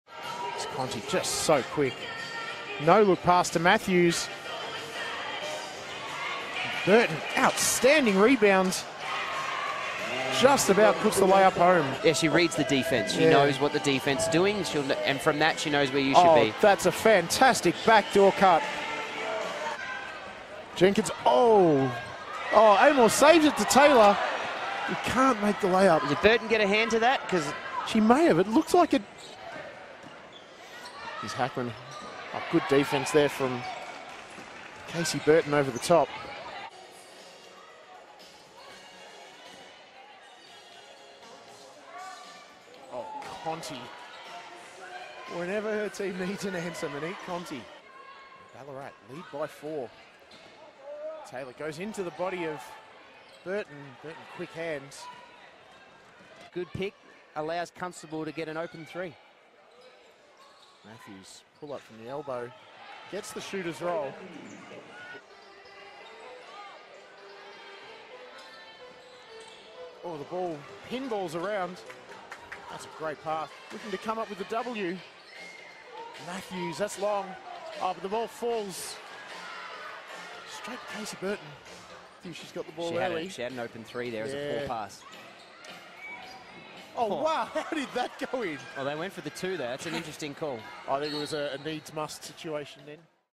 Kasey Burton Posts 11 points & 12 rebounds vs. Ballarat
Kasey Burton Posts 11 points & 12 rebounds vs. Ballarat, 07/20/2019